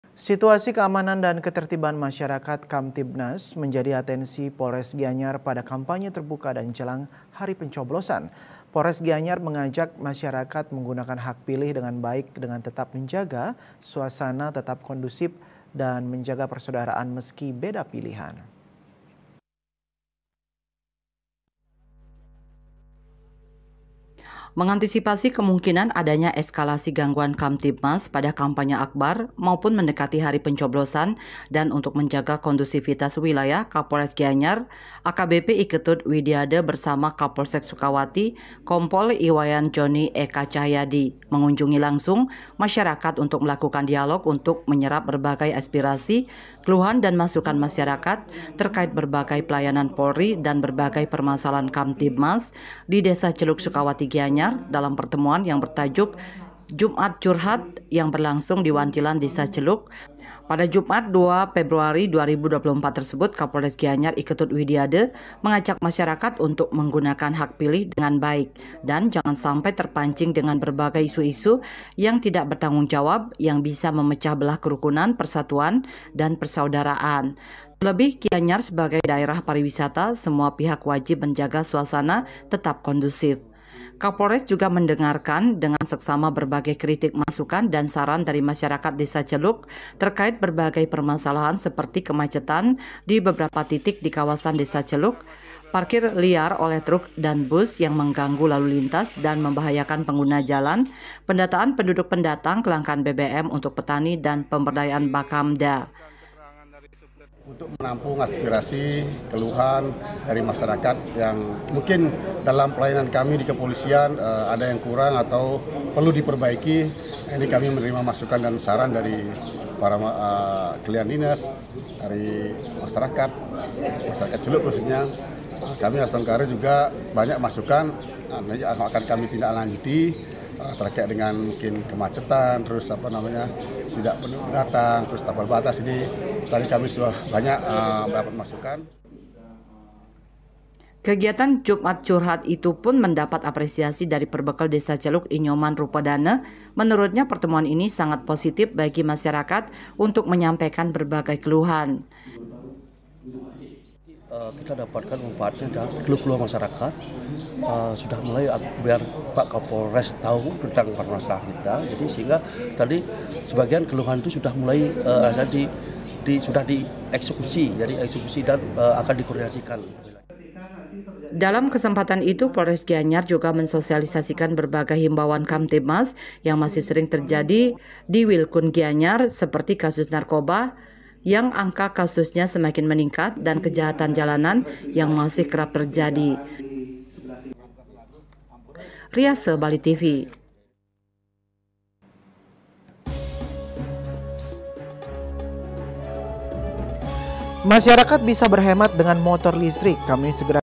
JAGA KAMTIBMAS JELANG PENCOBLOSAN, POLRES GIANYAR DATANGI MASYARAKAT
SITUASI KEAMANAN DAN KETERTIBAN MASYARAKAT ,KAMTIBMAS,MENJADI ATENSI POLRES GIANYAR PADA KAMPANYE TERBUKA DAN JELANG HARI PENCOBLOSAN. POLRES GIANYAR MENGAJAK MASYARAKAT MENGGUNAKAN HAK PILIH DENGAN BAIK DENGAN TETAP MENJAGA SUSANA TETAP KONDUSIF DAN MENJAGA PERSAUDARAAN MESKI BEDA PILIHAN.